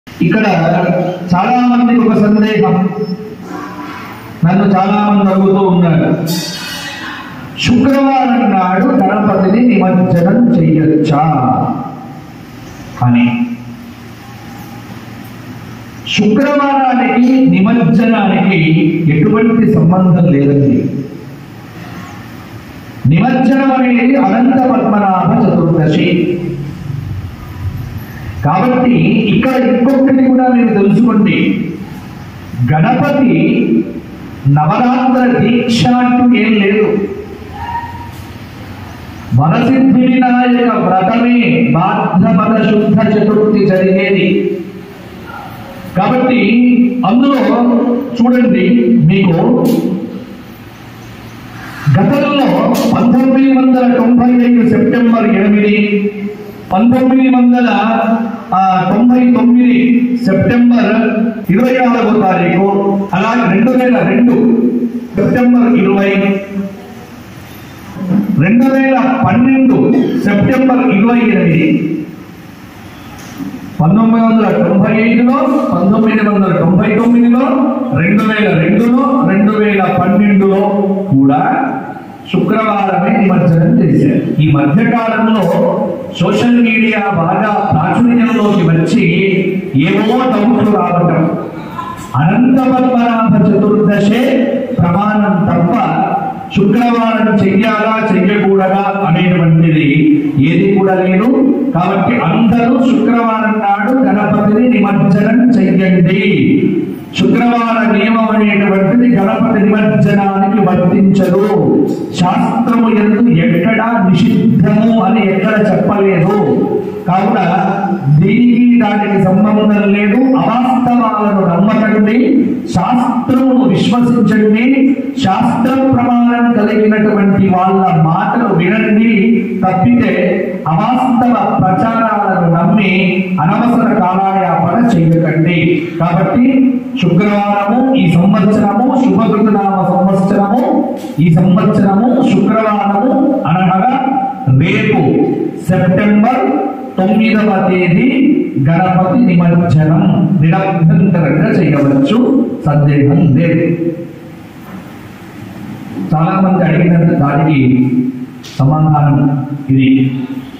09 సెప్టెంబర్ 2022 శుక్రవారం వినాయక నిమజ్జనం చేయవచ్చా.. కూడడా.. ఎందుకు..
09 సెప్టెంబర్ 2022 శుక్రవారం వినాయక నిమజ్జనం చేయవచ్చా.. కూడడా.. ఎందుకు.. by Dr Bachampally santosh kumar sastry